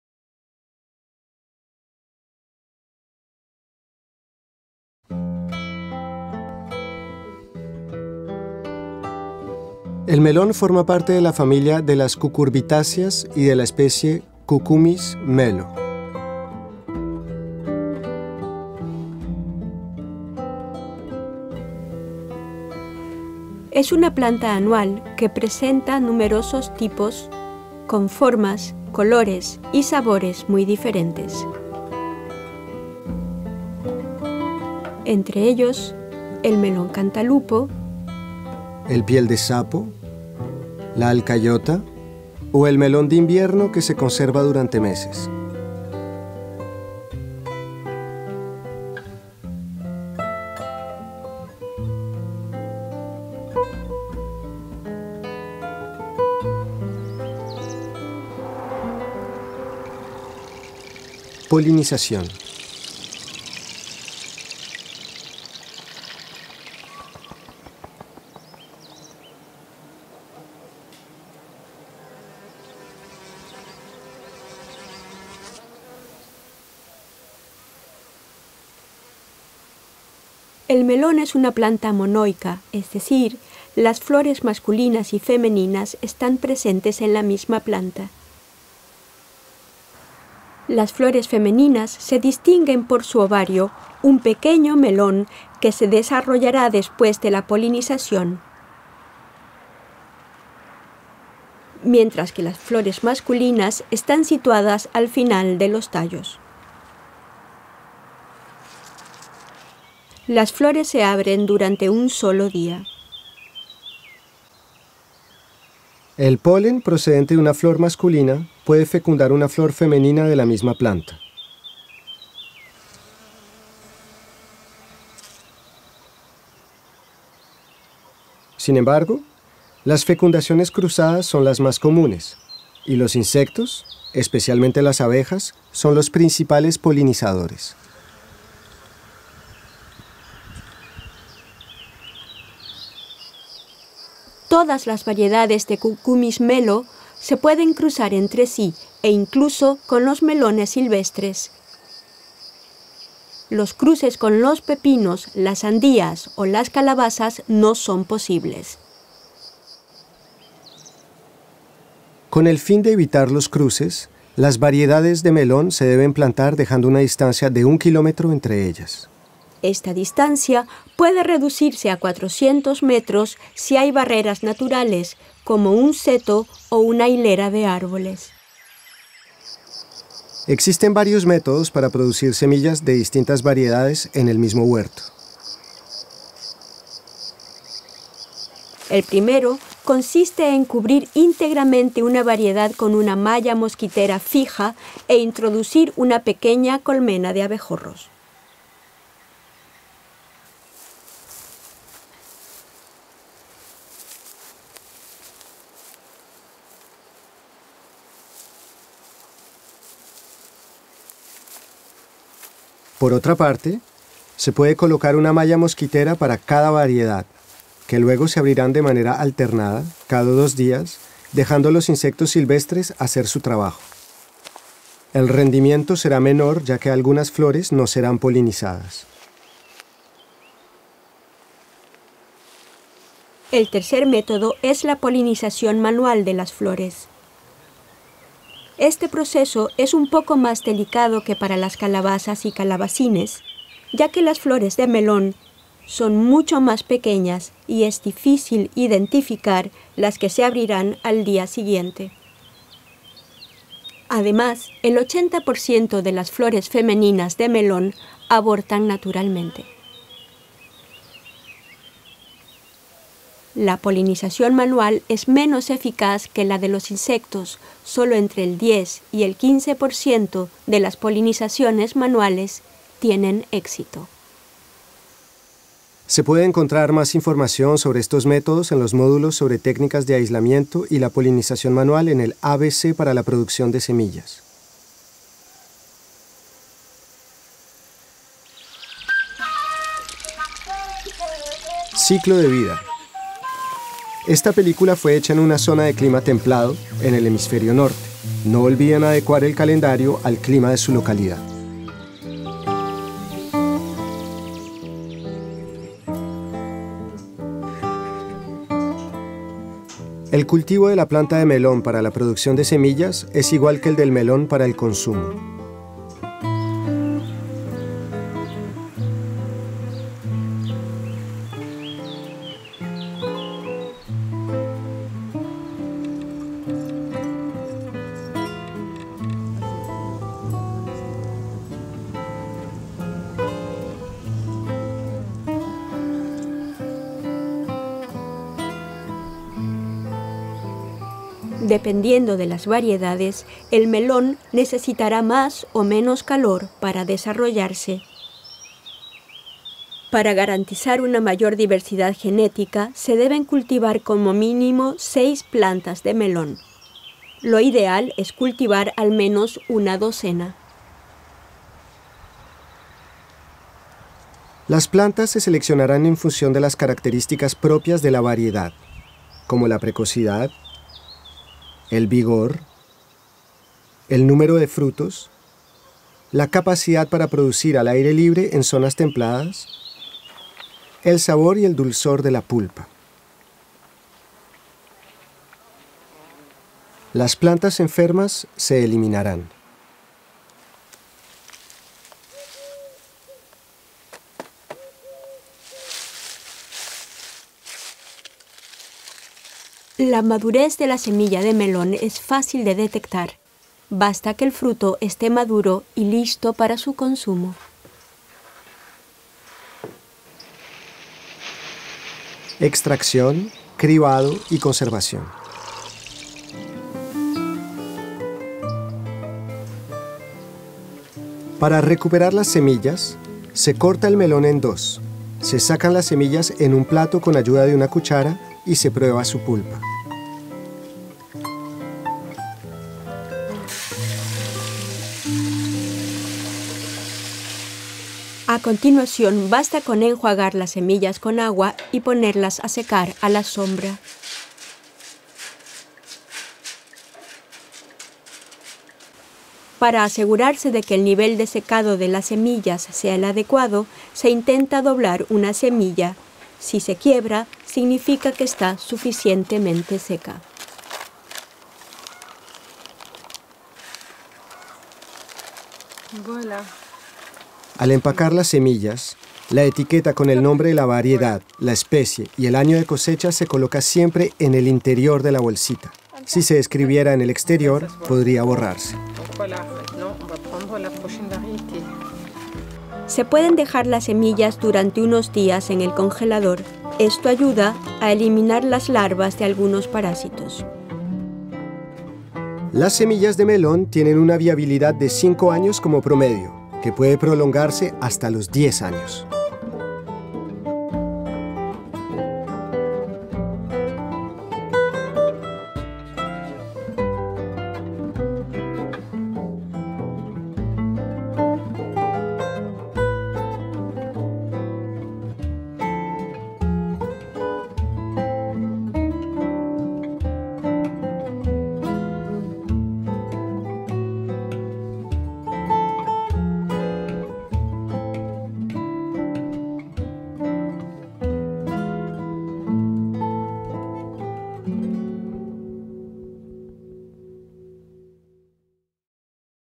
Cómo reproducir las semillas de melón
En este vídeo didáctico, podrás aprender cómo hacer tus propias semillas de melón. Podrás comprender su ciclo de vida, elegir y cultivar la planta que producirá las semillas, así como cosechar, seleccionar y conservar las semillas. Las explicaciones y las imágenes te mostrarán paso a paso todos los gestos y astucias necesarios.